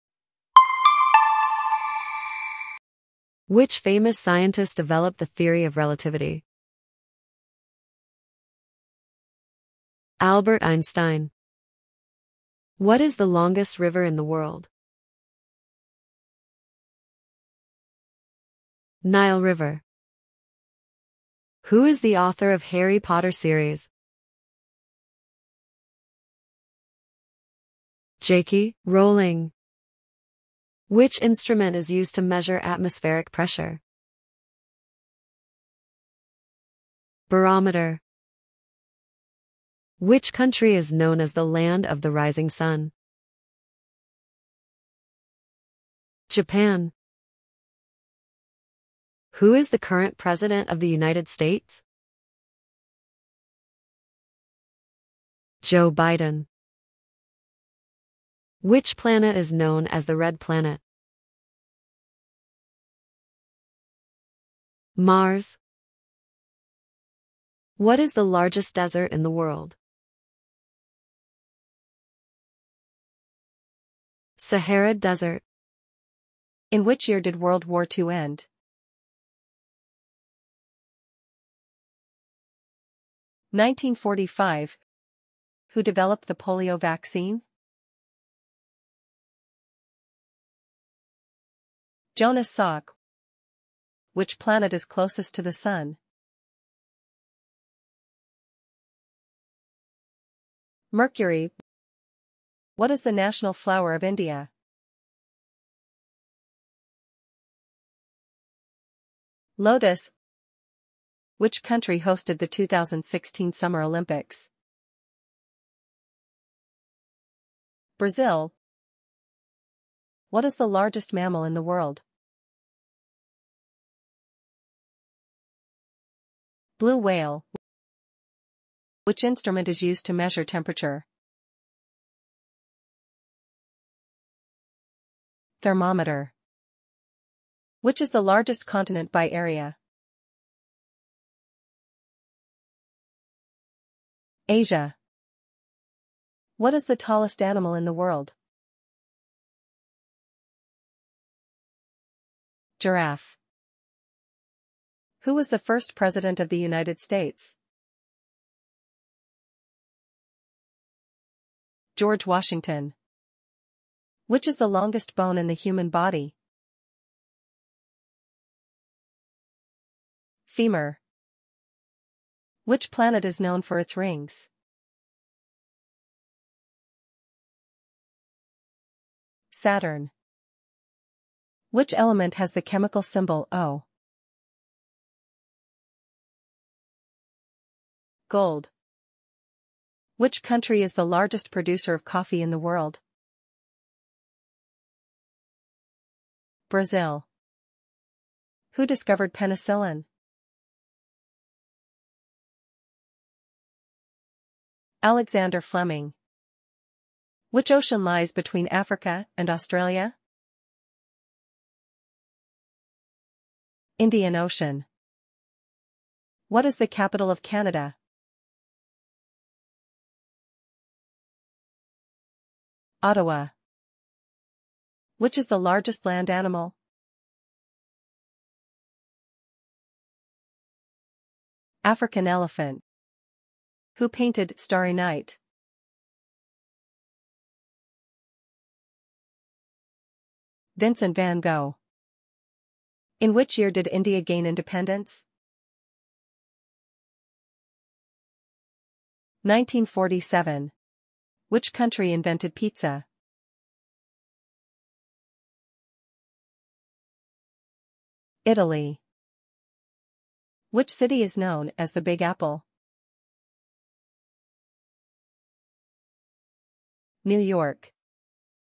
Think Fast: The Ultimate Knowledge Quiz | Part 12 | Fun Questions & Facts for Everyone
Challenge yourself with this fun and engaging general knowledge quiz!

From history and culture to geography and pop trivia, we've got a mix of questions that will test what you know. Whether you're a trivia buff or just curious to learn something new, this quiz is perfect for all ages.

How much do you really know?

Share your score in the comments!"